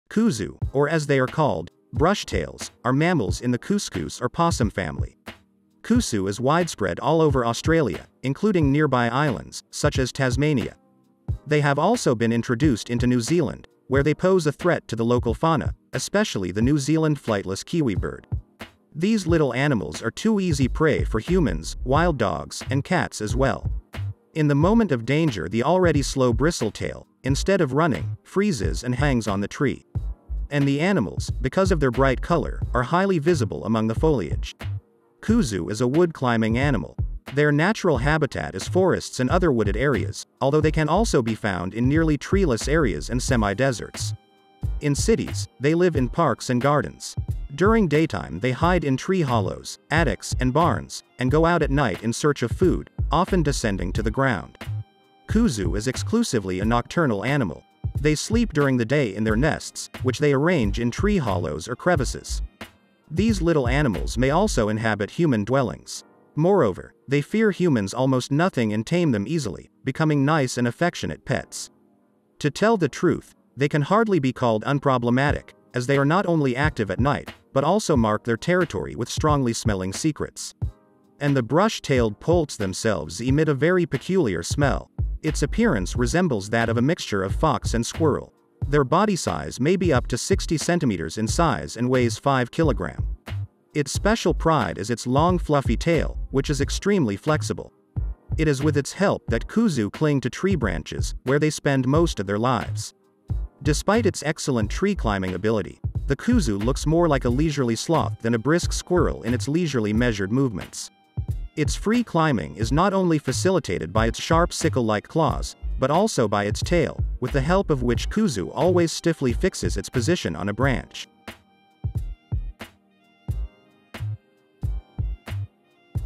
Amazing Facts About Kuzu fox - Interesting Facts Kuzu fox. Animals Life
Kuzu, or as they are called, brush-tails, are mammals in the couscous or possum family.

Kusu is widespread all over Australia, including nearby islands, such as Tasmania. They have also been introduced into New Zealand, where they pose a threat to the local fauna, especially the New Zealand flightless kiwi bird.

These little animals are too easy prey for humans, wild dogs, and cats as well. In the moment of danger the already slow bristle-tail, instead of running, freezes and hangs on the tree. And the animals, because of their bright color, are highly visible among the foliage.

Kuzu is a wood climbing animal. Their natural habitat is forests and other wooded areas, although they can also be found in nearly treeless areas and semi-deserts. In cities, they live in parks and gardens. During daytime they hide in tree hollows, attics, and barns, and go out at night in search of food, often descending to the ground.

Kuzu is exclusively a nocturnal animal. They sleep during the day in their nests, which they arrange in tree hollows or crevices. These little animals may also inhabit human dwellings. Moreover, they fear humans almost nothing and tame them easily, becoming nice and affectionate pets. To tell the truth, they can hardly be called unproblematic, as they are not only active at night, but also mark their territory with strongly smelling secrets. And the brush-tailed poults themselves emit a very peculiar smell.